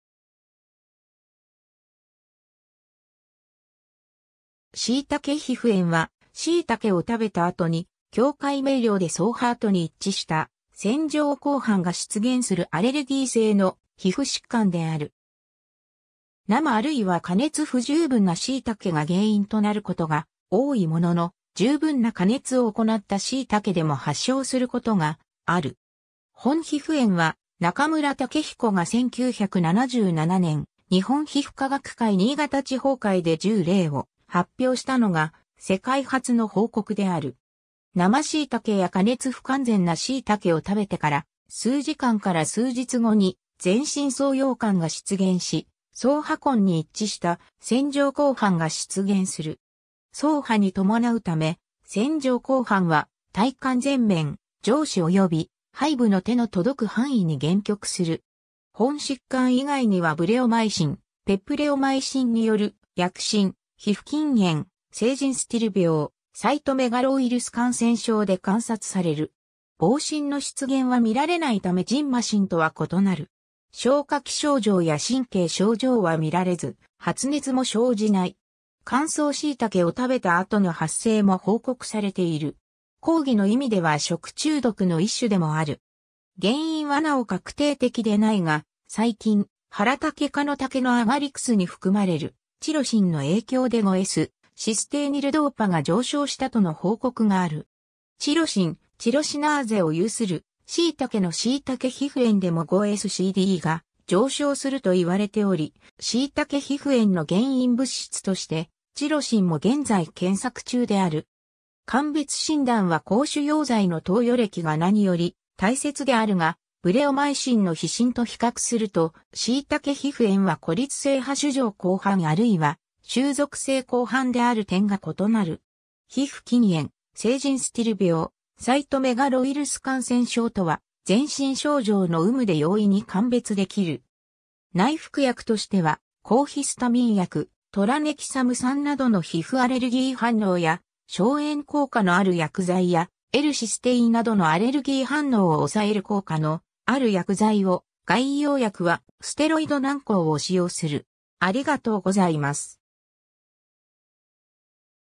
しいたけ皮膚炎
しいたけ皮膚炎（しいたけひふえん）は、シイタケを食べた後に境界明瞭で掻爬跡に一致した線状紅斑が出現するアレルギー性の皮膚疾患である。
生あるいは加熱不充分なシイタケが原因となることが多いものの、充分な加熱を行ったシイタケでも発症することがある。
 本皮膚炎は、中村雄彦（たけひこ）が1977年、日本皮膚科学会新潟地方会で10例を発表したのが世界初の報告である。
生シイタケや加熱不完全なシイタケを食べてから数時間から数日後に、全身掻痒感が出現し、掻破痕に一致した線状紅斑が出現する。
掻爬に伴うため、線状紅斑は体幹前面・上肢および背部の手の届く範囲（上背部と腰部）に限局する。
本疾患以外にはブレオマイシン/ペプレオマイシンによる薬疹、皮膚筋炎、成人スティル病、サイトメガロウイルス感染症で観察される。
膨疹の出現はみられないため蕁麻疹とは異なる。
消化器症状や神経症状はみられず、発熱も生じない。
乾燥シイタケを食べた後の発生も報告されている。
広義の意味では食中毒の1種でもある。
 原因はなお確定的でないが、最近ハラタケ科の茸のアガリクスに含まれるチロシンの影響で5-S-システイニルドーパ（5-S-CD）が上昇したとの報告がある。
チロシン、チロシナーゼを有するシイタケのシイタケ皮膚炎でも5-S-CDが上昇するといわれており、シイタケ皮膚炎の原因物質としてチロシンも現在検索中である。
 鑑別診断は抗腫瘍剤の投与歴がなにより大切であるが、ブレオマイシンの皮疹と比較すると、シイタケ皮膚炎は孤立性播種状紅斑あるいは集簇性紅斑である点が異なる。
皮膚筋炎、成人スティル病、サイトメガロウイルス感染症とは、全身症状の有無で容易に鑑別できる。
 内服薬としては、抗ヒスタミン薬、トラネキサム酸などの皮膚アレルギー反応や消炎効果のある薬剤や、L-システインなどのアレルギー反応を抑える効果のある薬剤を、外用薬はステロイド軟膏を使用する。